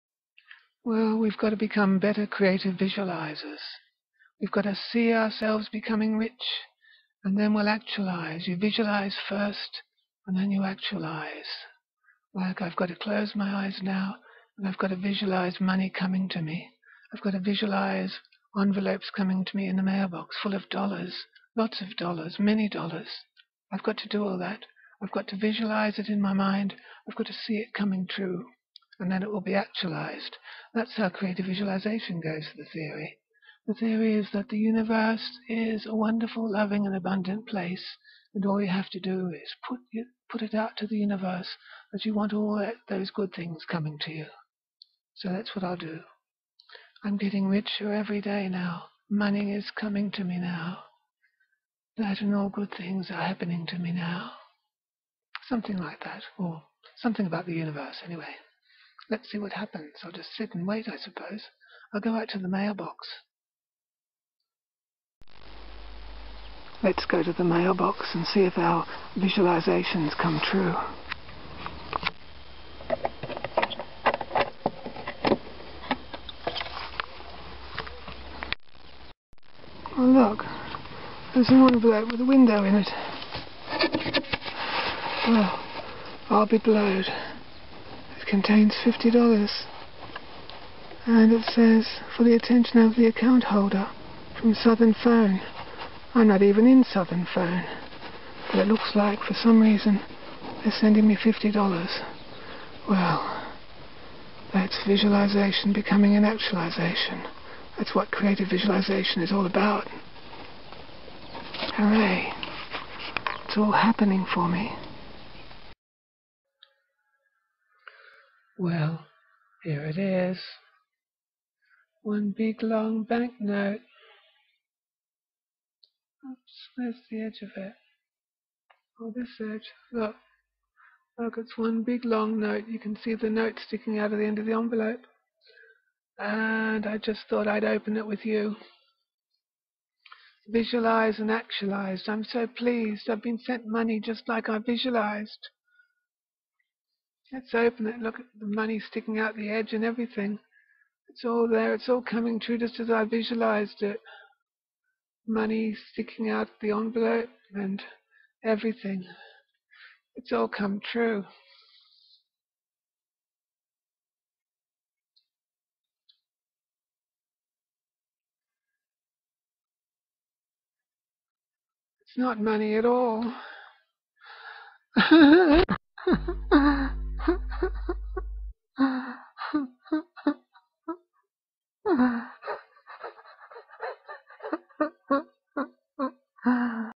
Creative Visualization and Money ◔_◔ $
Well, the Creative Visualization theory goes that what one visualizes, one actualizes. Here's me imaging money coming to me. I go out to my mailbox to see it all coming true. I visualized money coming to me in an envelope. I then find an envelope with a $50 dollar bill inside--from Southern Phone. All for me! How thoughtful of them!